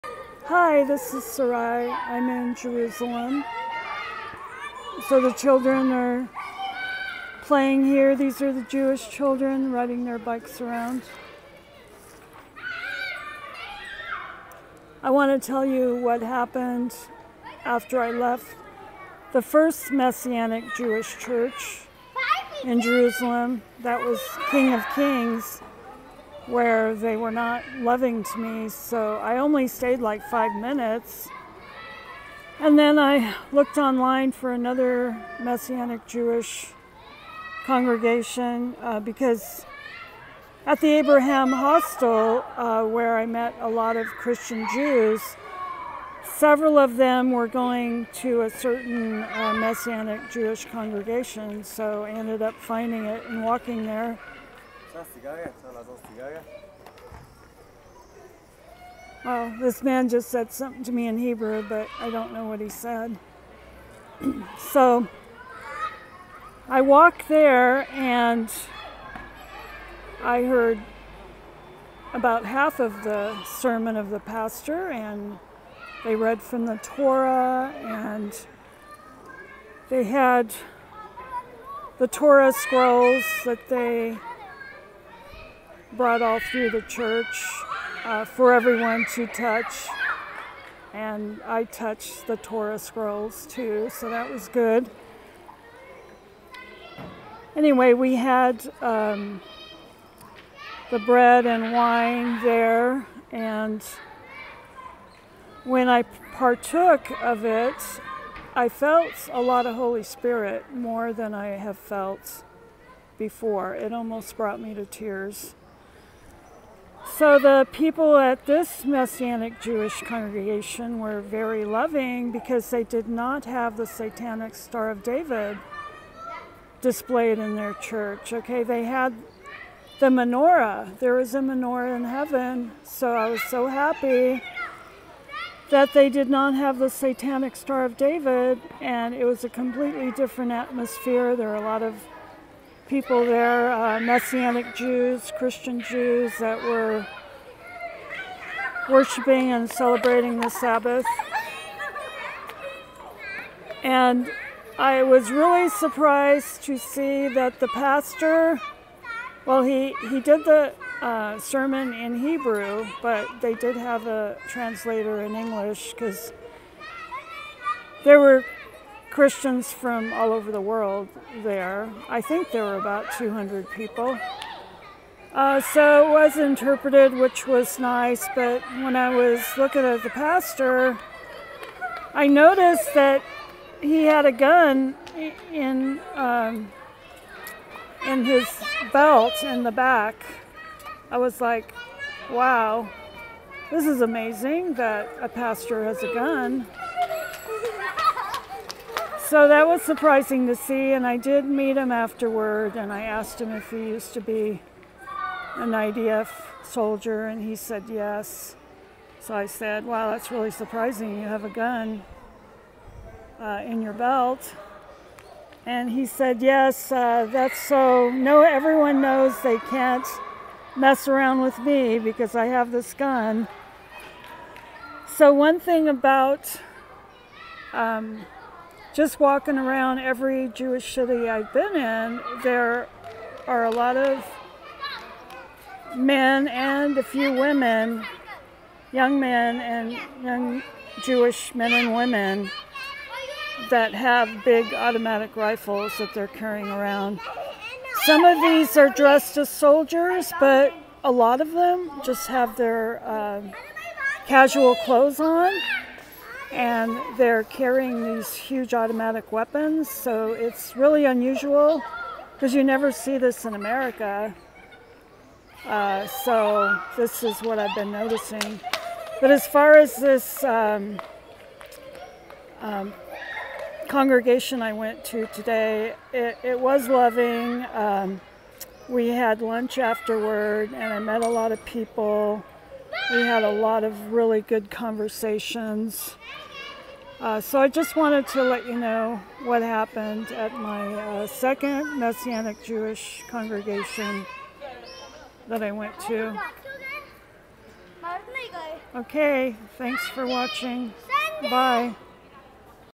Messianic Jews Were Loving At The Sabbath Service/Pastor Has Gun In His Belt ￼
At the second Messianic Jewish congregation that I went to in Jerusalem, the Christian Jews were nice and loving. I felt a lot of Holy Spirit when I partook of the wine and bread representing the blood of Jesus poured out for us and his body broken for us as a sacrifice for our sins. ￼